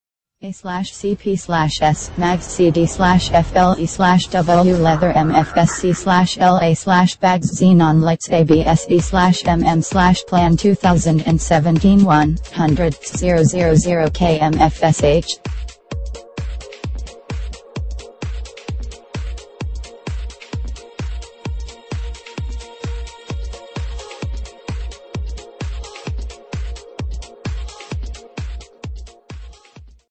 2013 AUDI A4 1.8T S 88KW
A/c, p/s, mags, cd/fl, e/w, leather, MFS, c/l, a/bags, xenon lights, ABS, e/m, m/plan 2017/100 000km, FSH!